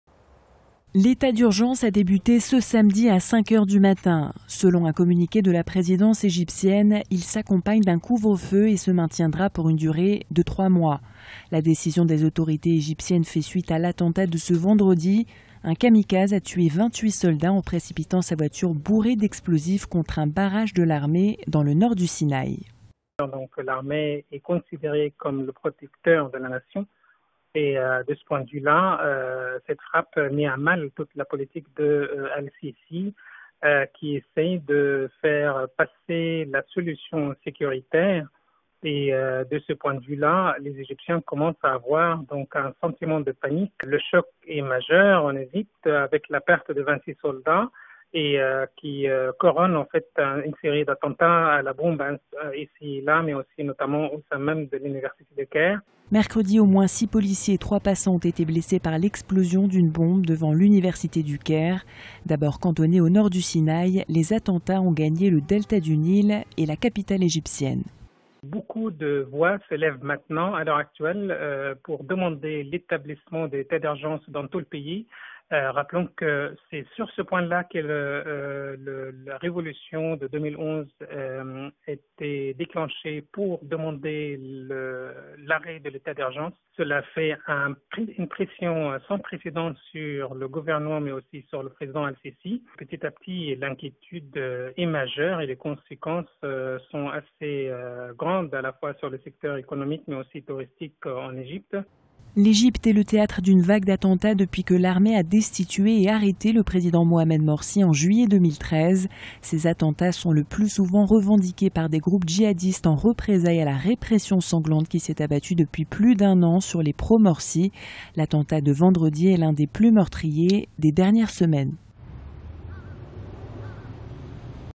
Égypte, L'Etat d'urgence inquiète
Politique, Egypte: L'Egypte décrète l'état d'urgence sur une partie du nord du Sinaï / Conséquence de l'attaque contre 25 soldats. Mohammed Abdel Azim, Analyste politique, évoque les conséquences politiques et économiques de cette situation. 15h08